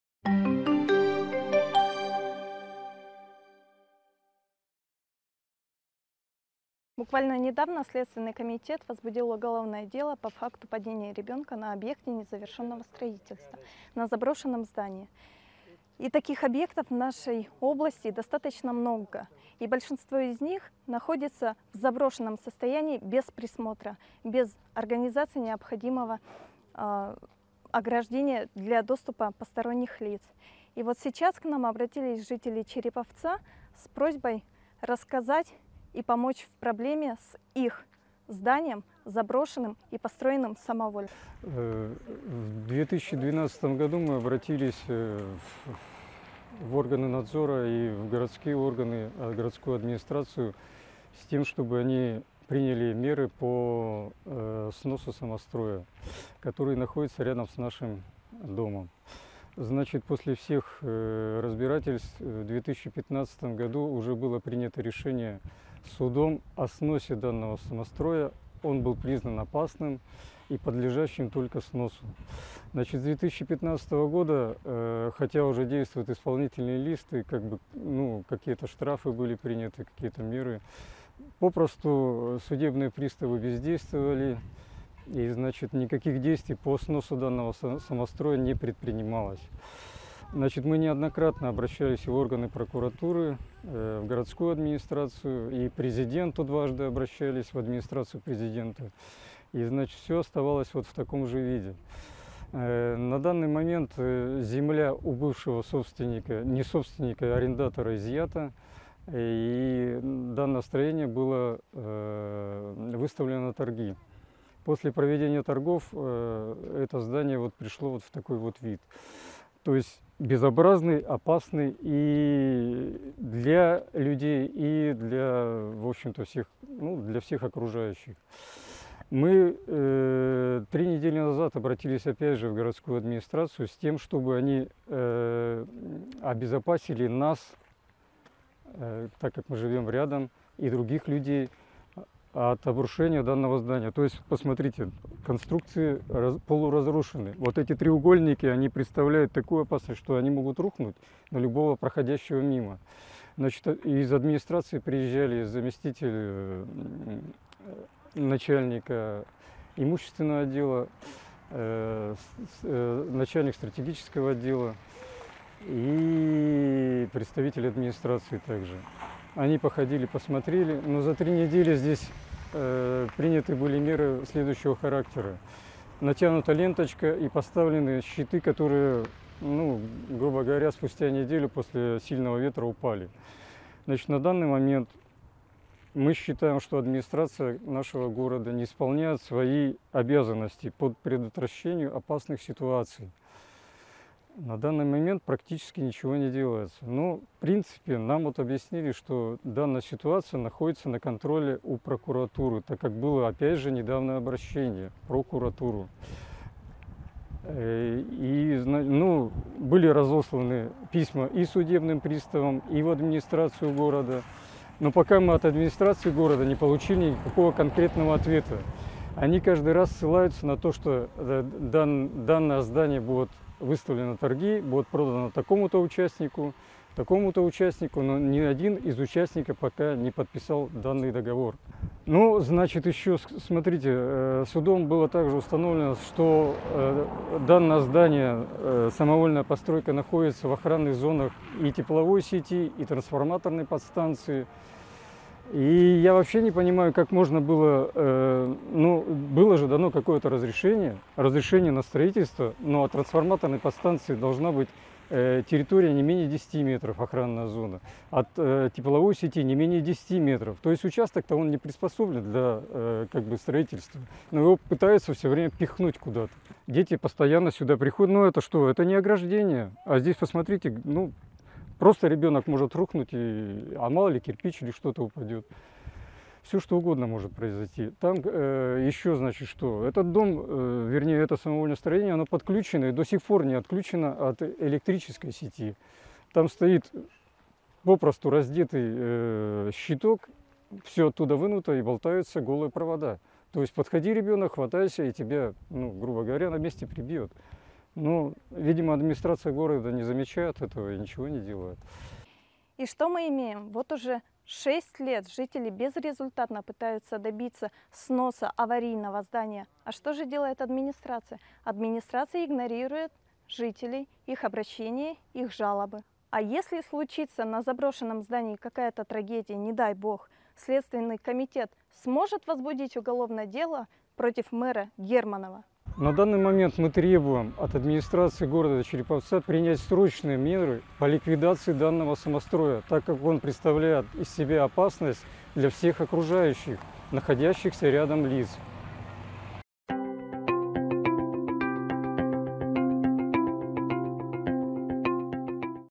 Опасный самострой - Многомэрская история
На днях Следственное управление Вологодской области объявило о возбуждении уголовного дела по факту падения ребенка на стройке. И тут же к нам обратились жители Череповца много лет обеспокоенные опасным самостроем под своими окнами. Оказалось, что стройка начатая 12 лет назад, ещё при мэре Кувшинникове, была в 2015 году признана судом самостроем и подлежала сносу. Однако, за эти годы сменилось три мэра, а решение суда так и не выполнено. При этом доступ на опасный самострой открыт и на нём часто играют дети. Мы очень хотим предотвратить очередную трагедию